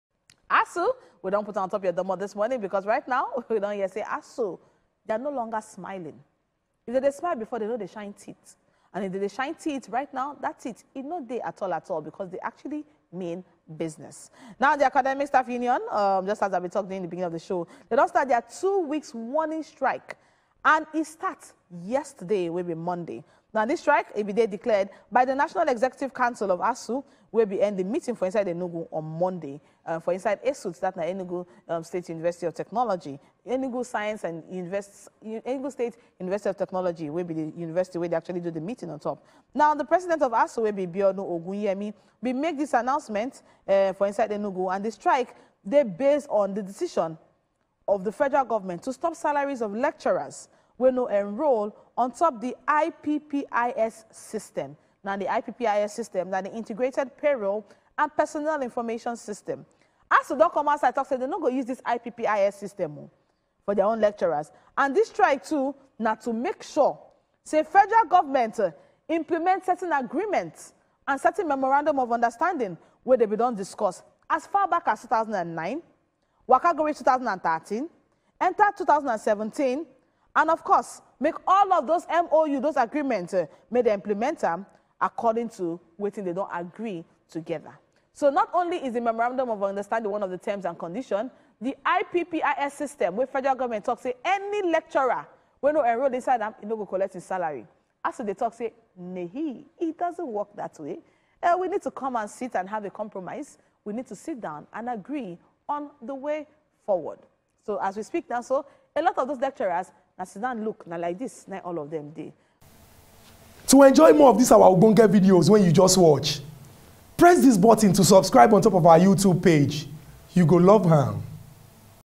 ASUU COMMENCES NATIONWIDE STRIKE - wazobiamax
The Academic Staff Union of Universities (ASUU) has embarked on a two-week warning strike with effect from Monday.

The strike was declared by the National Executive Committee (NEC) of ASUU which ended its meeting in Enugu on Monday.

The President of ASUU, Biodun Ogunyemi, announced the strike in Enugu.